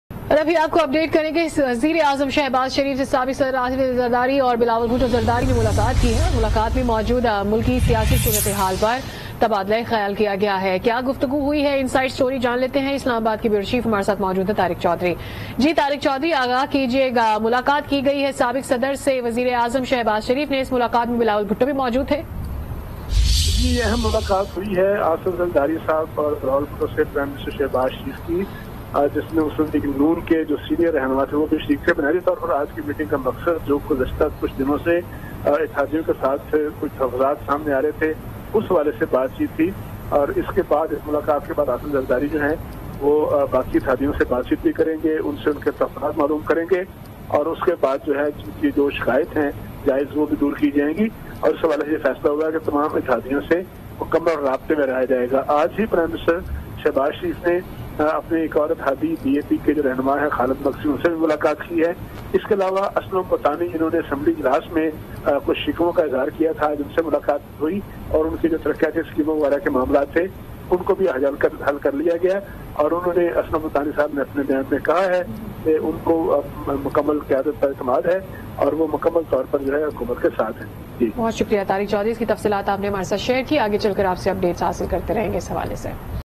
PM Shehbaz Sharif say Asif Zardari aur Bilawal Bhutto ki mulaqat | Aaj Updates
Aaj TV Official:
shorturl.at/iBDNX